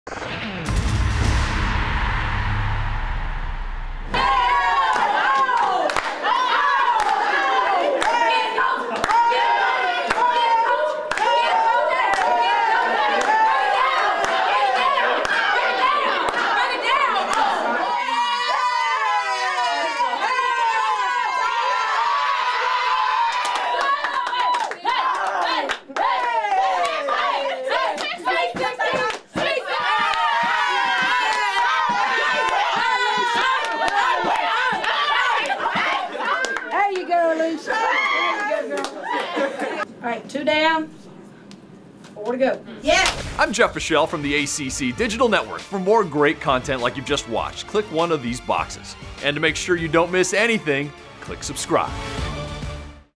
UNC's Locker Room Celebrates After Buzzer Beater | ACC Must See Moment
North Carolina head coach Sylvia Hatchel celebrates with her team after the buzzer beater win over Ohio State 86-84.  Watch the team get ready for their Sweet 16 dance with a locker room dance party in this ACC Must See Moment!  The video was provided by GoHeels.com.